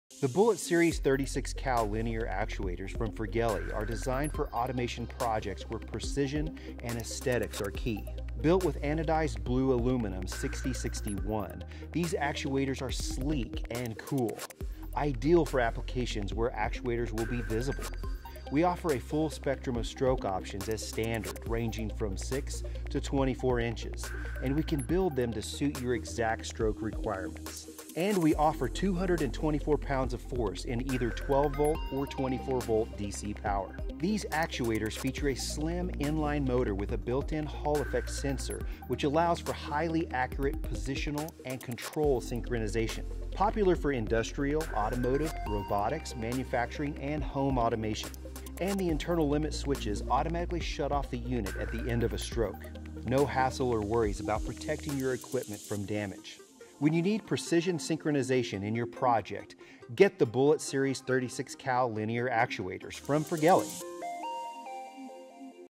Guess who won the global Actuator Design Competition - Firgelli Won
Isn't it time someone made a really cool looking Linear Actuator ? we thought so too.

Using our corporate colors we created a Blue anodized line of Linear Actuators. Available in different sizes, voltages, forces and speeds, we are sure these Actuators will suit just about any application.

Nick-named the Bullet Actuator, due to their shape we are excited to get this into the hands of customers.